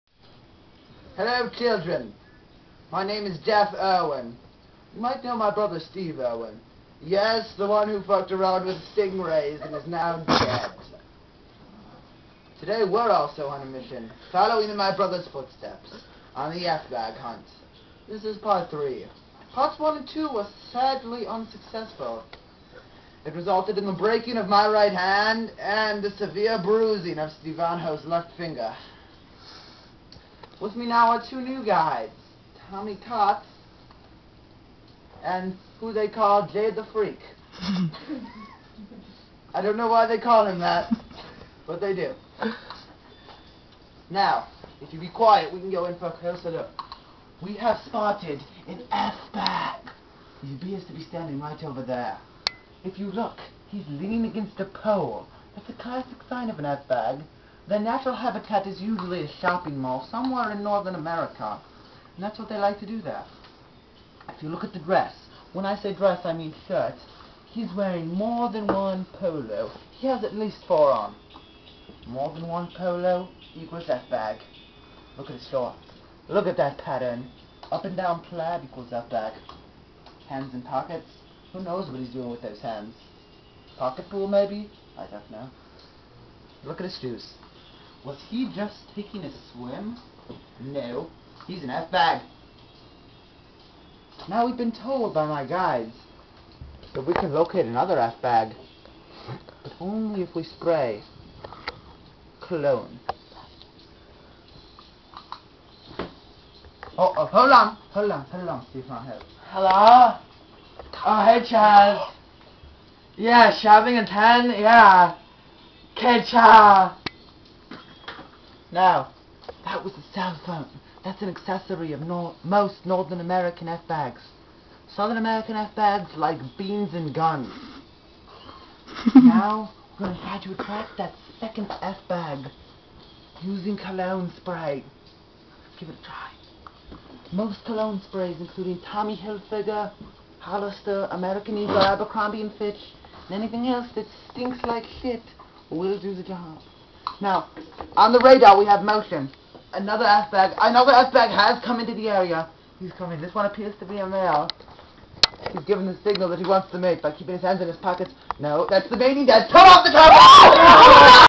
The Hunt
Ahhhh, The F Bag hunt continues, Jeff Irwin is back with Steve-hano and 2 new guides on another F-Bag adventure!